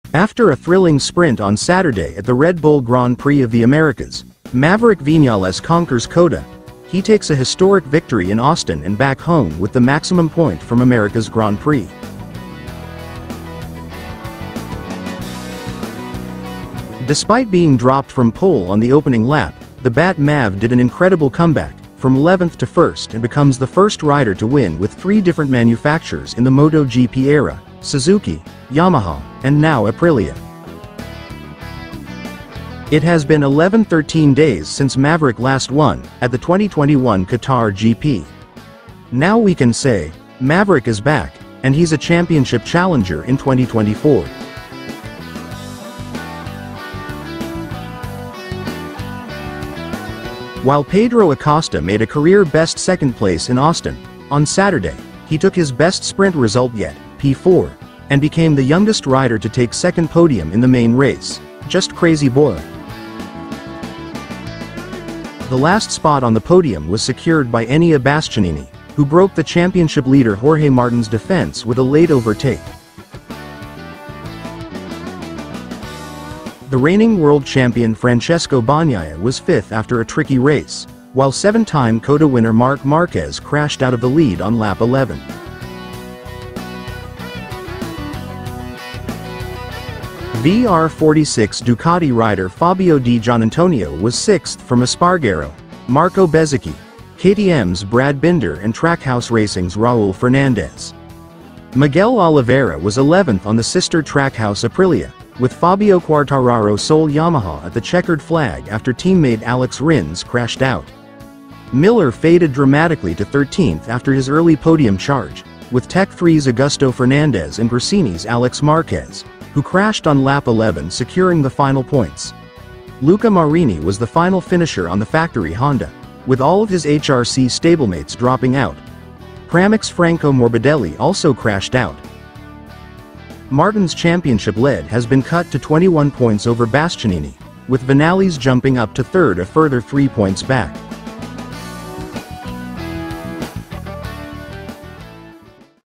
UNBELIEVABLE!!! Maverick Viñales Broke New Record in Americas MotoGP Race CoTA 2024
MotoGP America 2024